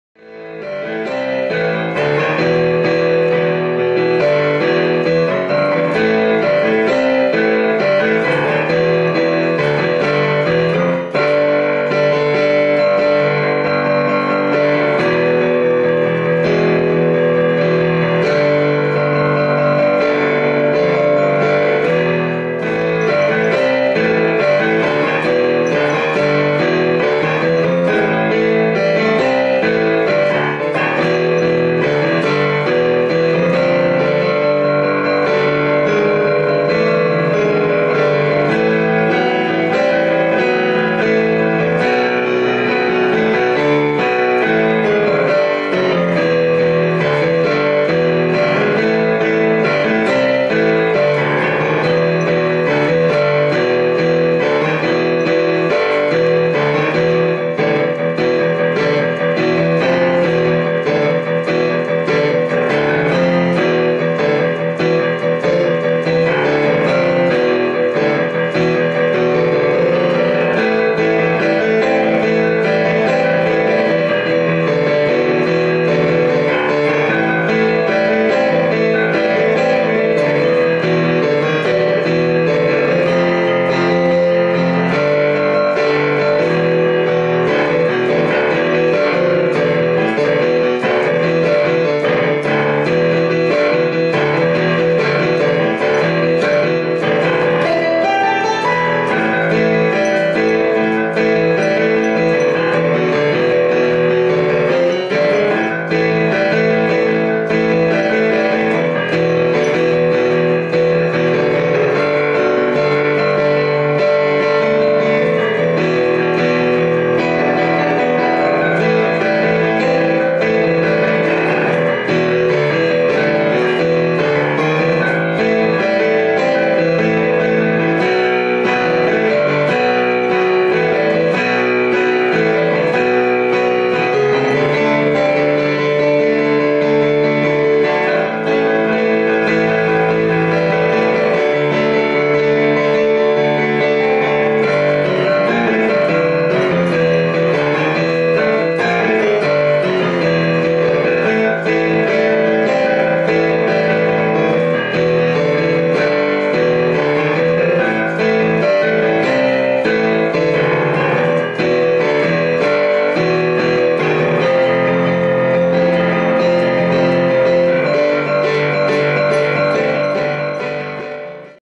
5 30 13 Piano Jam a hard rock Idea
Writing session coming up with new ideas for songs.